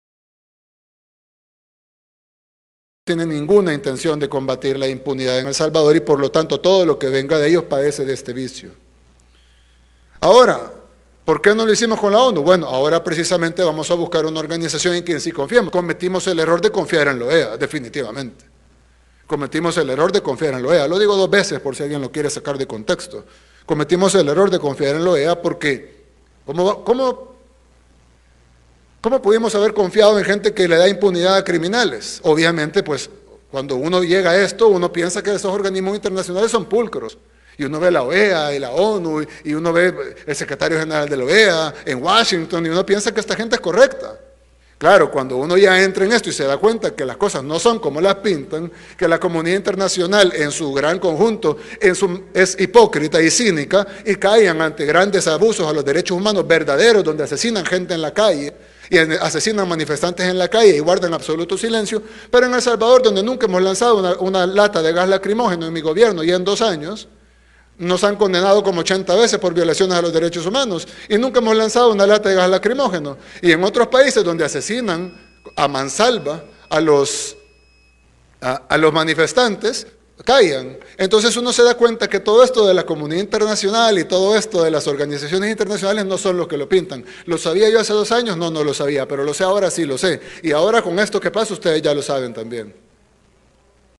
NayibBukele: “Cometimos el error de confiar en la OEA” más info en descripción.
El Secretario general de la OEA, Luis Almagro, nombró como asesor de la secretaría general de la OEA a Ernesto Muyshandt, justo un día antes de la audiencia judicial de éste, por motivo de corrupción. Esto provocó que NAYIB BUKELE, presidente de El Salvador, rompiera el convenio con la OEA sobre la lucha contra la impunidad que había hecho con la organización. “Obviamente es un acto de impunidad nombrar en la OEA a alguien que tiene arresto domiciliario y audiencia judicial justo al día siguiente de su nombramiento.” Dijo Nayib Bukele.